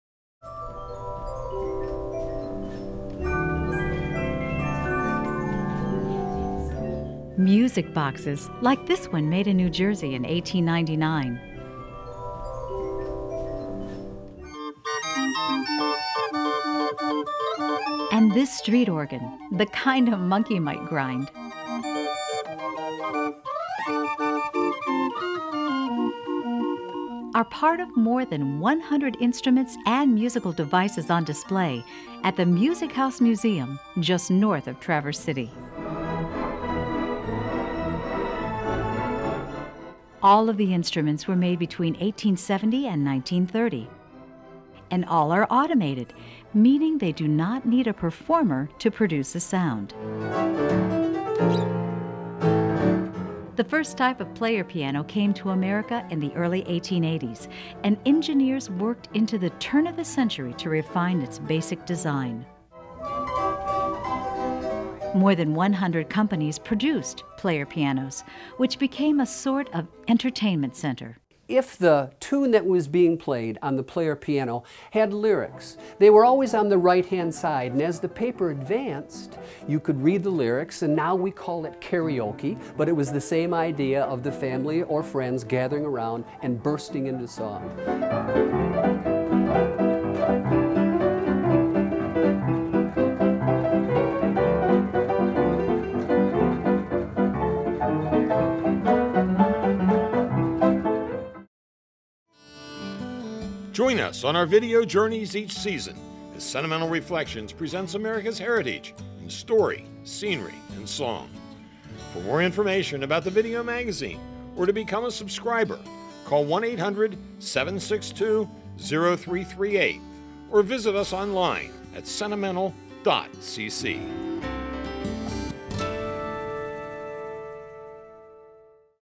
Sentimental Reflections Music House Museum
Sentimental Reflections is a quarterly video series produced by Sentimental Productions that celebrates America's heritage in story, scenery and song. This clip features the Music House Museum near Traverse City, Michigan.  It is the On A Musical Note segment in the Summer 2010 Edition of the video magazine.